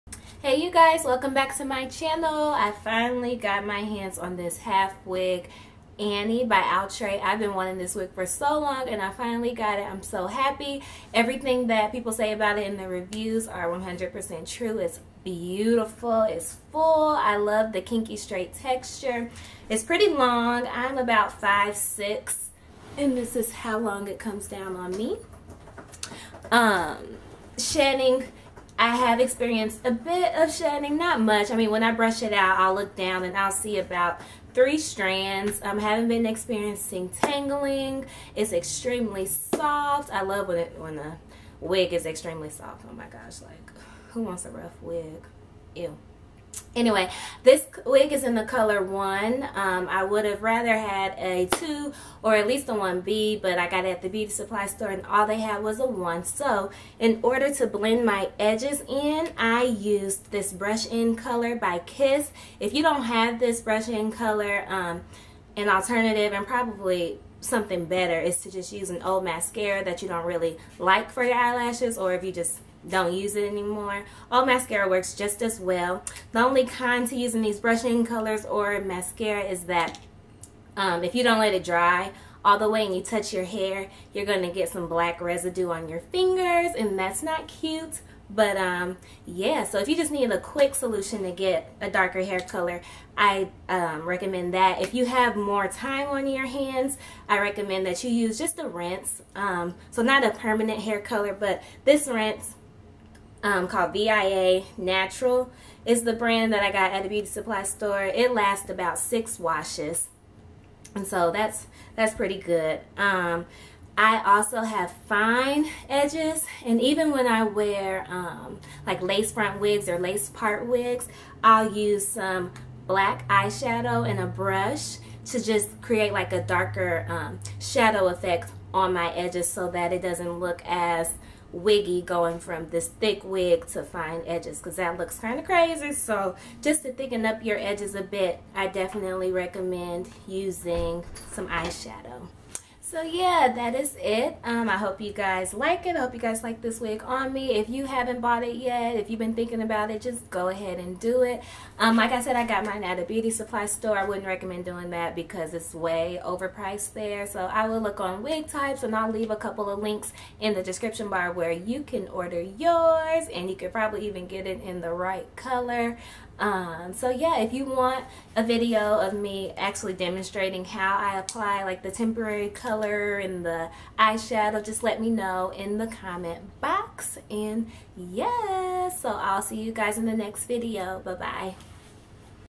Outre Annie : How to make your edges appear darker and fuller
Show and Tell of the popular halfwig Annie by Outre
What I use to make my edges appear darker an fuller for better blending.